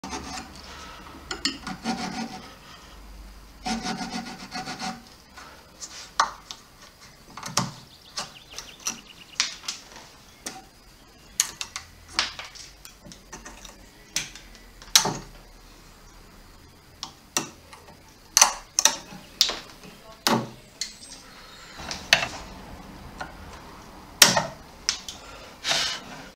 Star roughing
Freeing the turn of the scroll with saw cuts